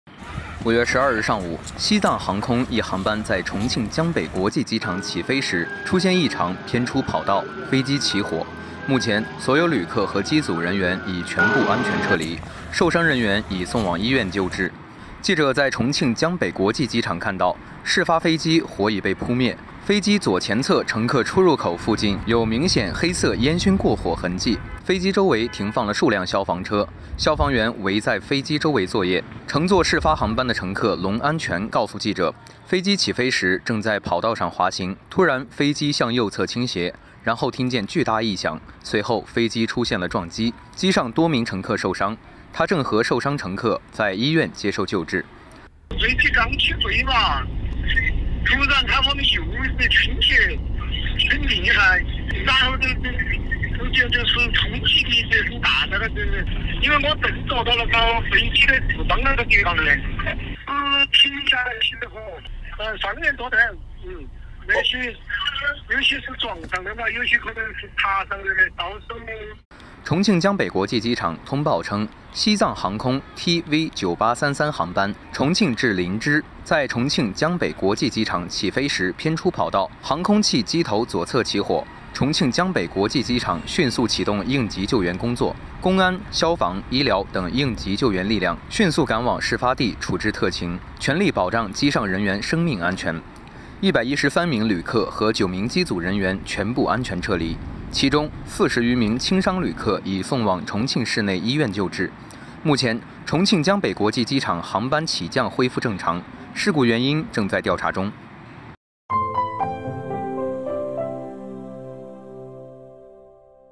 重庆机场一客机偏出跑道起火 乘客称曾听见异响
5月12日上午，西藏航空一航班在重庆江北国际机场起飞时出现异常偏出跑道，飞机起火。目前，所有旅客和机组人员已全部安全撤离，受伤人员已送往医院救治。
        记者在重庆江北国际机场看到，事发飞机火已被扑灭，飞机左前侧乘客出入口附近有明显黑色烟熏过火痕迹。飞机周围停放了数辆消防车，消防员围在飞机周围作业。
        乘坐事发航班的乘客龙安全告诉记者，飞机起飞时正在跑道上滑行，突然飞机向右侧倾斜，然后听见巨大异响，随后飞机出现了撞击，机上多名乘客受伤，他正和受伤乘客在医院接受救治。